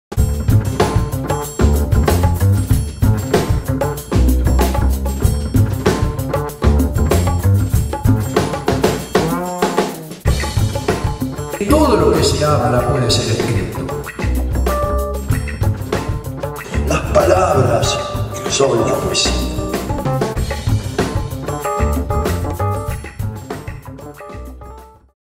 Alejandró Dolina participó del CILE
Alejandro Dolina también estuvo presente en el panel "El valor del español como lengua de culturas. Literatura, oralidad, folclore" en el marco del VIII Congreso Internacional de la Lengua Española.